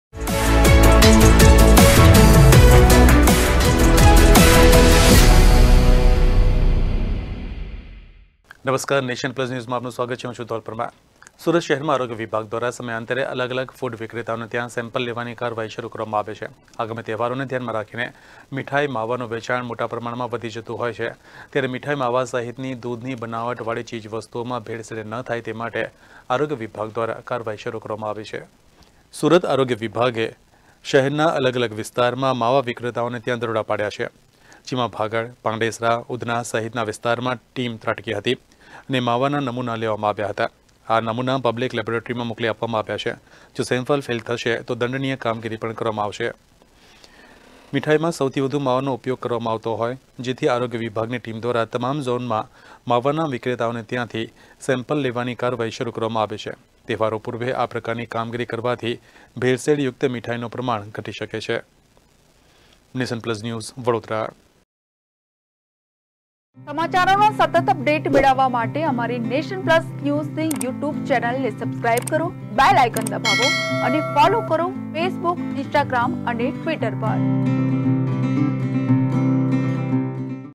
સુરતમાં આરોગ્ય વિભાગના દરોડા
સુરત શહેરમાં આરોગ્ય વિભાગ દ્વારા સમયાંતરે અલગ અલગ ફૂડ વિક્રેતાઓને ત્યાં સેમ્પલ લેવાની કાર્યવાહી શરૂ કરવામાં આવે છે. આગામી તહેવારોને ધ્યાનમાં રાખીને મિઠાઈ-માવાનું વેચાણ મોટા પ્રમાણમાં વધી જતું હોય છે. ત્યારે મિઠાઈ-માવા સહિતની દૂધની બનાવટ વાળી ચીજ-વસ્તુઓમાં ભેળસેળ ન થાય તેના માટે આરોગ્ય વિભાગ દ્વારા કાર્યવાહી શરૂ કરવામાં આવી છે.